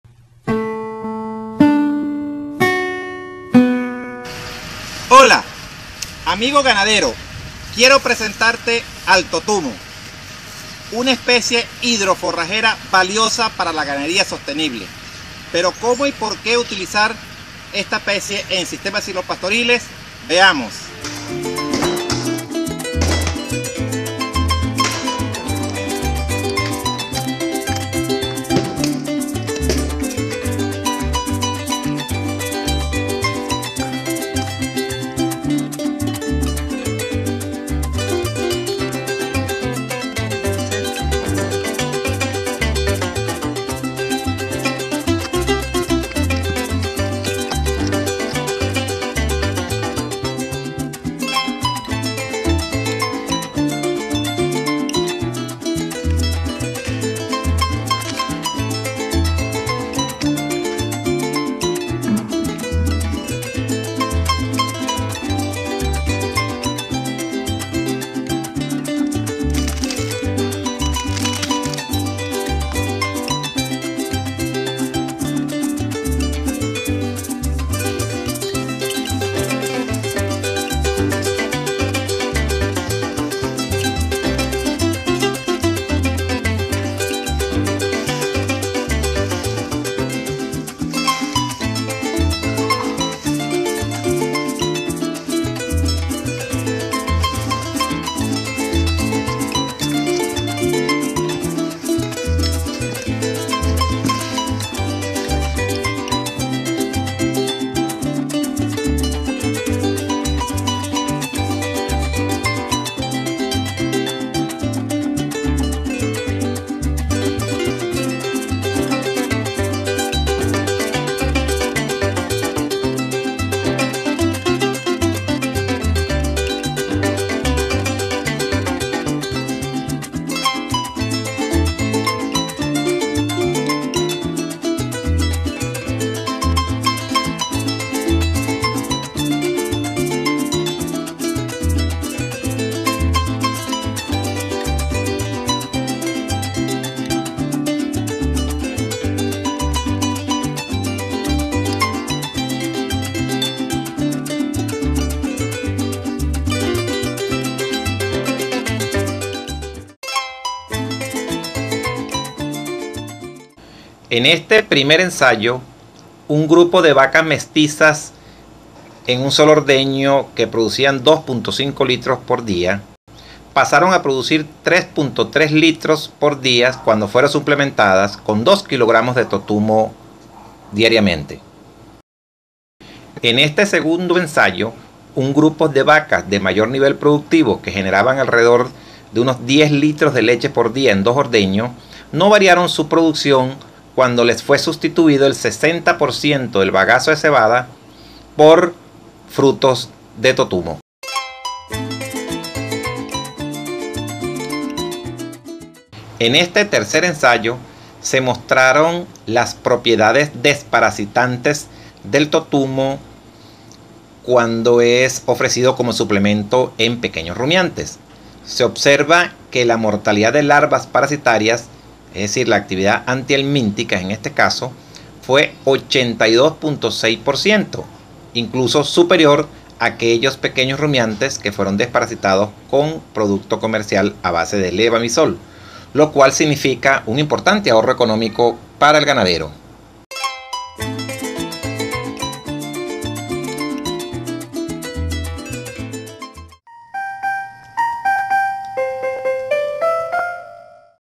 11 El Totumo como HidroForraje
El video muestra el Totumo (Crescentia cujete) como árbol forrajero. Sus frutos de ensilan y ofrecen forraje nutritivo y agua al ganado y demás animales domésticos.